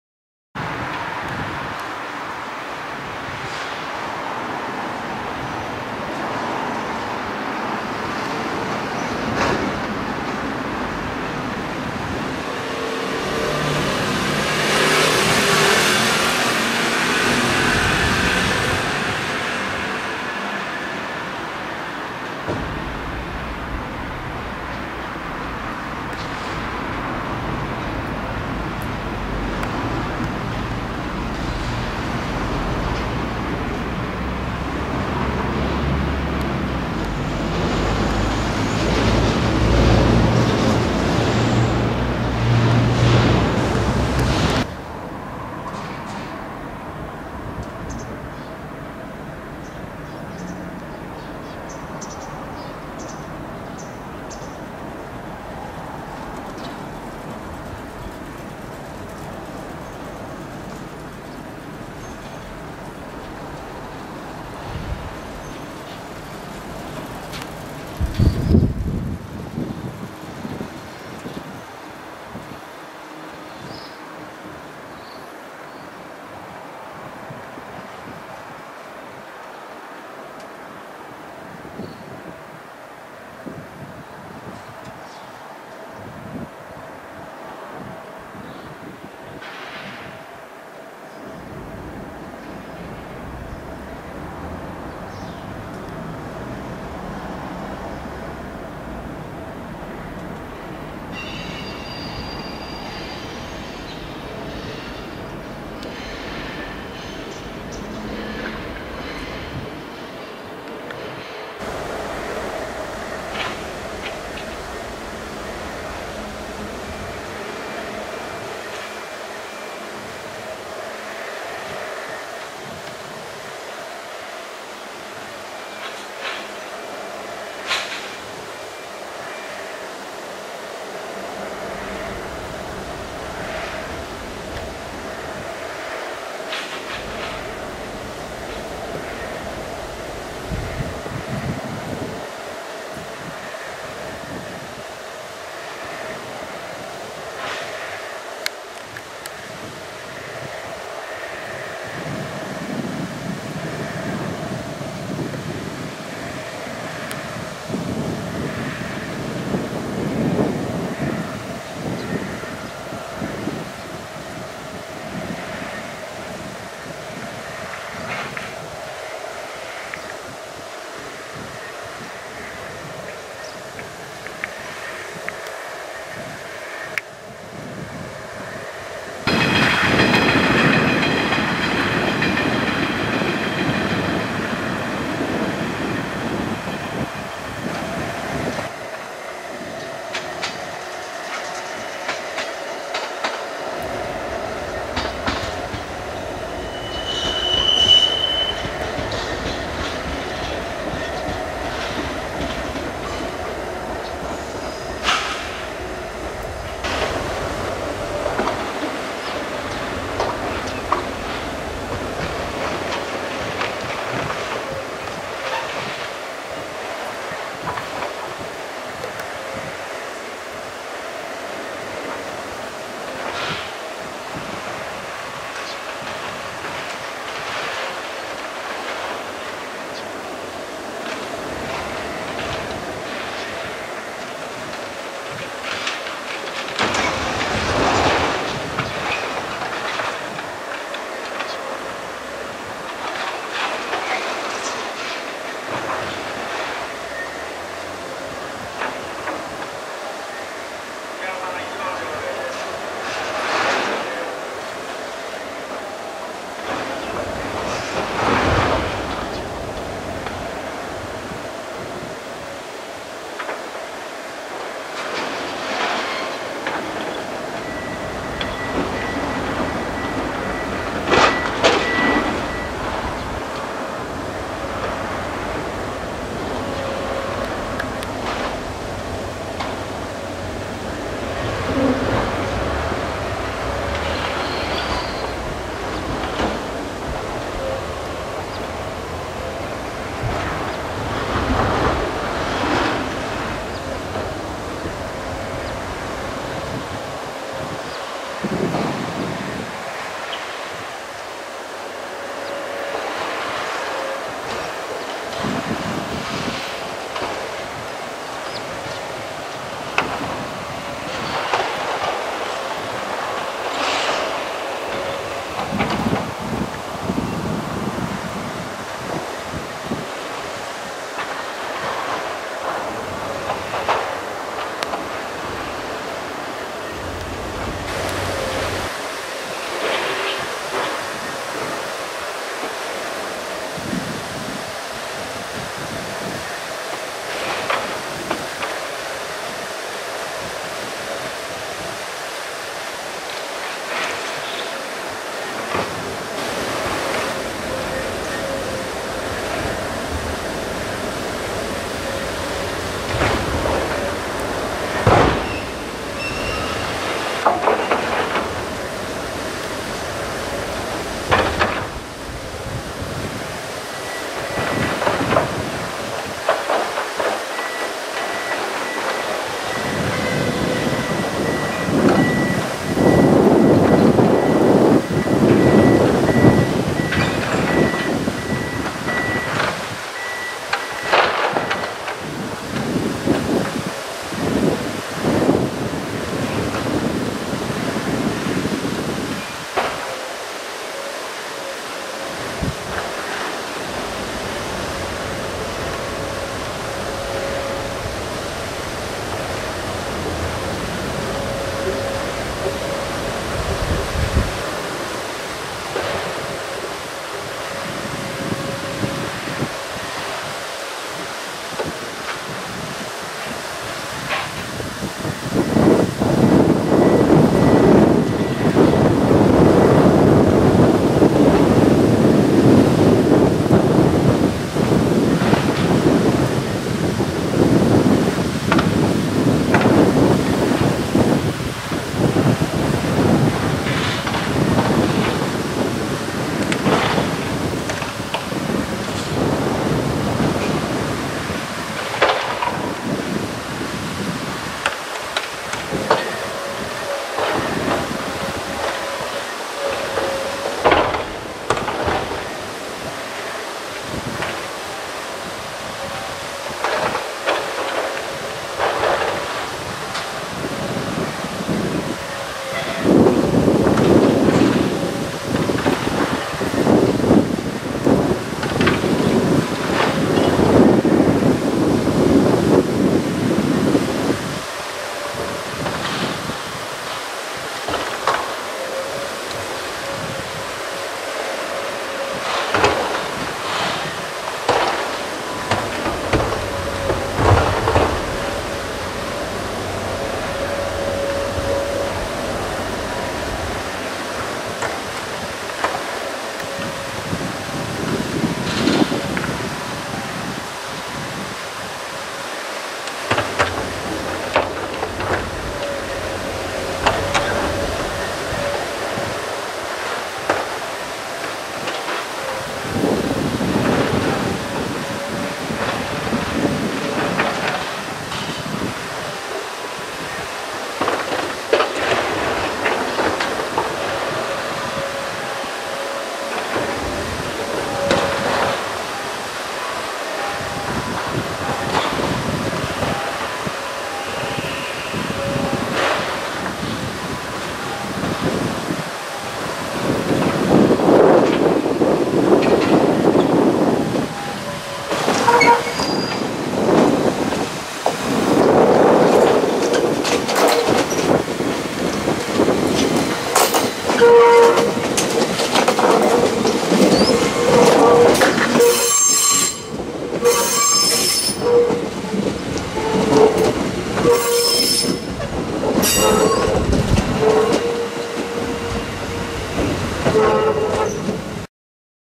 解体が始まった485系｢なのはな｣、長野総合車両センター。
10月24日(月)午前の、長野総合車両センター様子。
モロ484が、重機により車両解体をされてました。